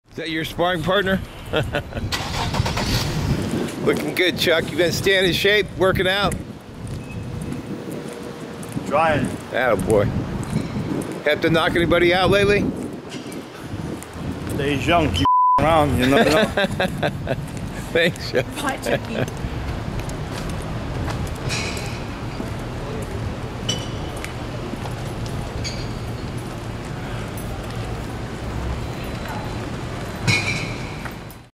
Tough Guy Chuck Zito Gets Lunch With Stallone
Saturday, January 7, 2017: Chuck Zito joked about keeping in shape and knocking people out, after lunch with friends; Sylvester Stallone, Ralf Moller and Mickey Rourke, at Cafe Roma in Beverly Hills. X17online.com  - Censored Language -